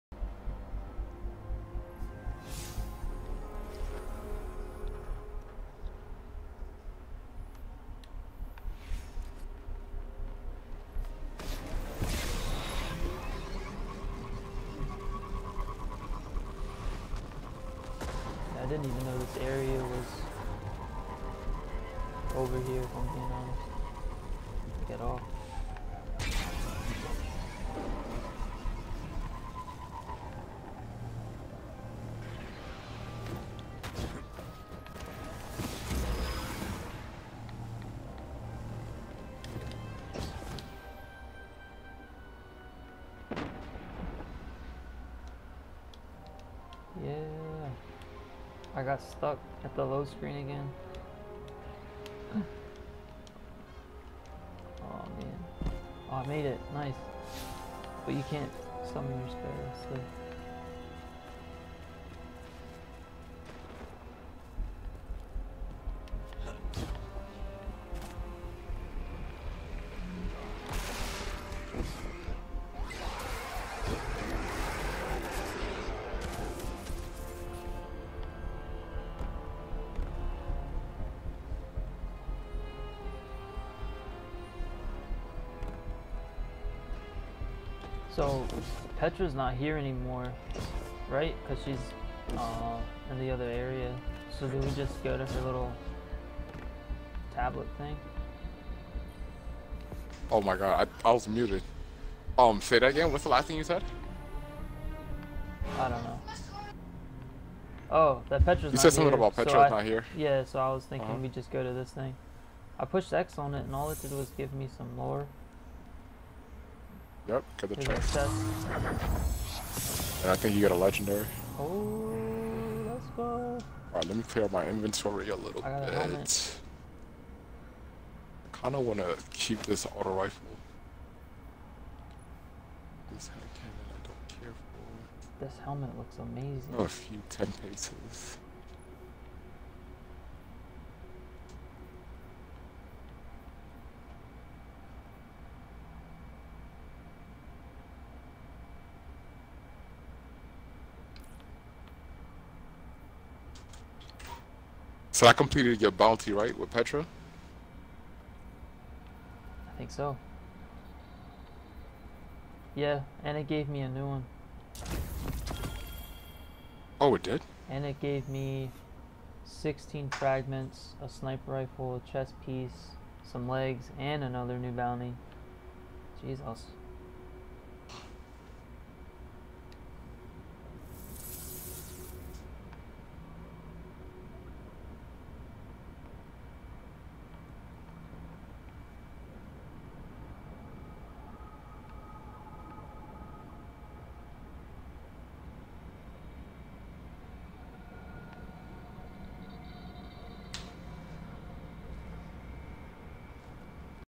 Audience with the Queen OFFERING LOCATION
This is where you turn in your offering after completing the quest. You get 3 drops. 1 was a basic drop and 2 were powerful drops.